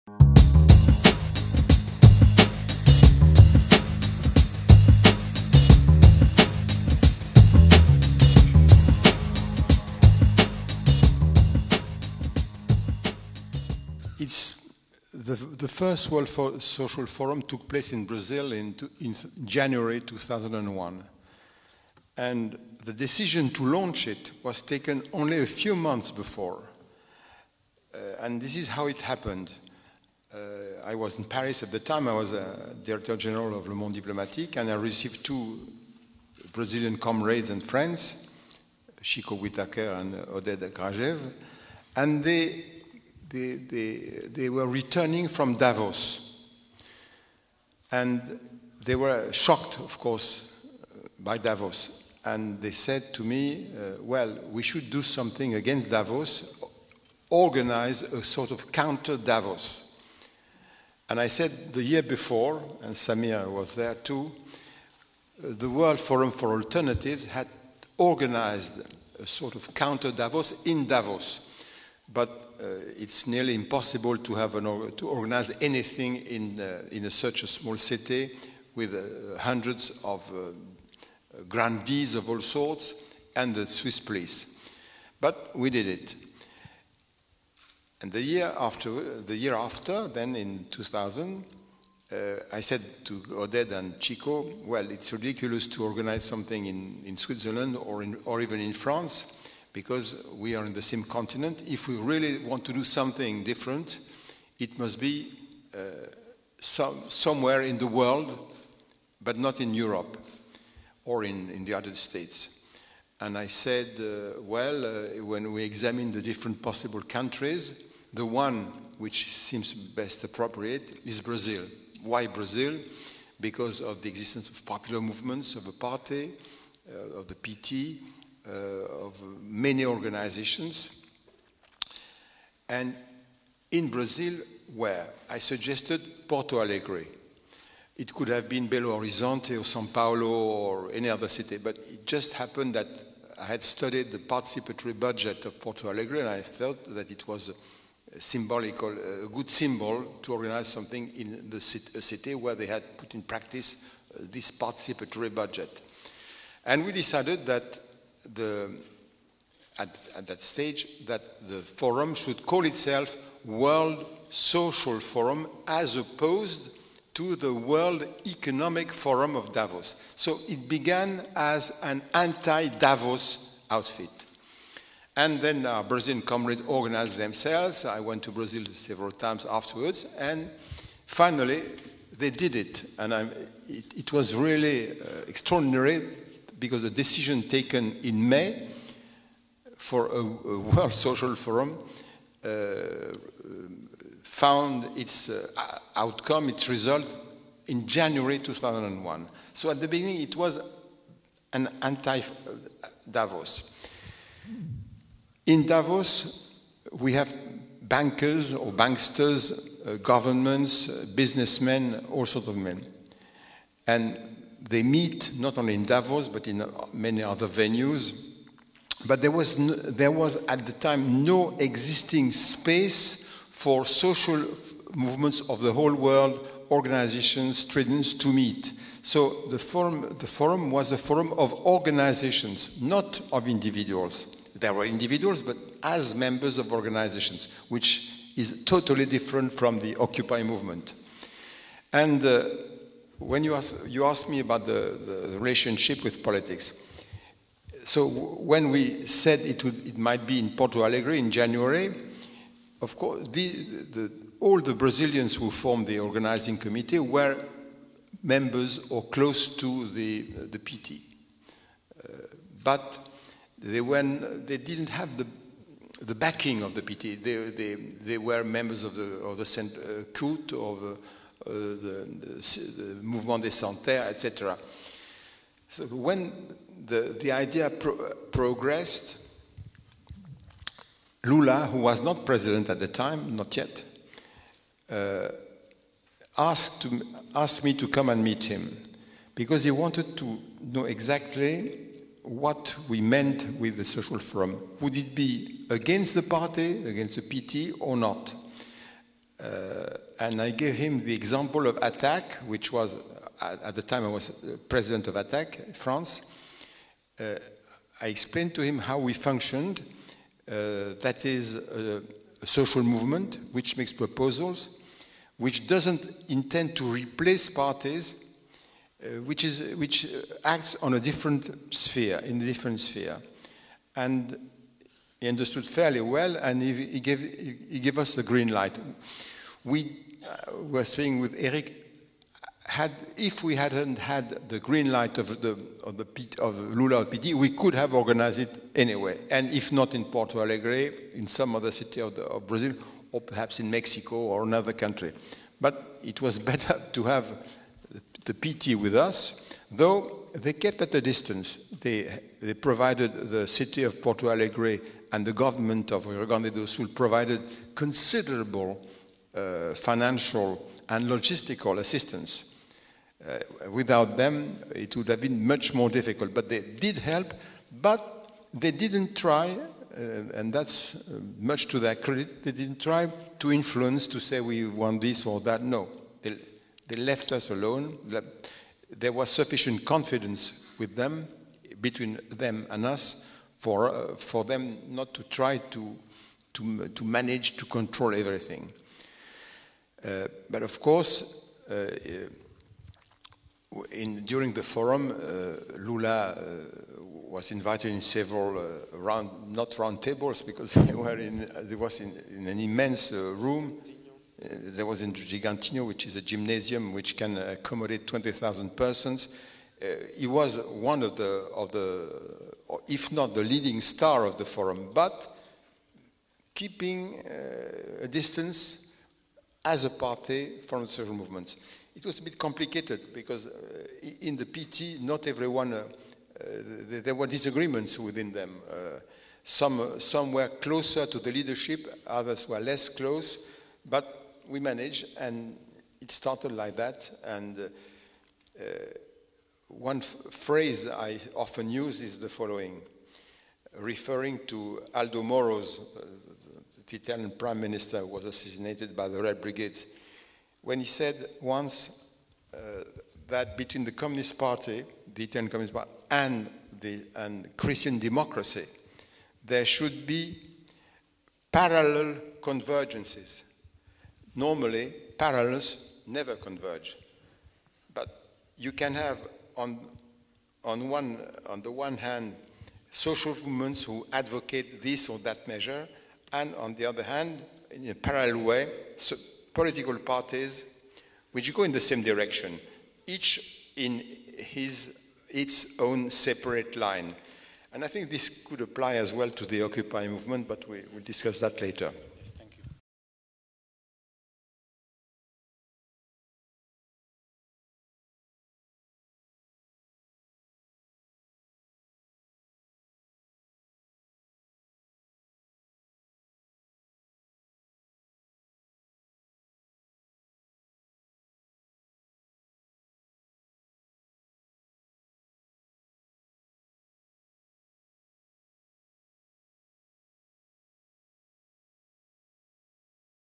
Bernard Cassen - The History of the World Social Forum
[En] Bernard Cassen on the history of the World Social Forum from Davos to Porto Allegre. The talk was held at the 5th Subversive Festival in Zagreb.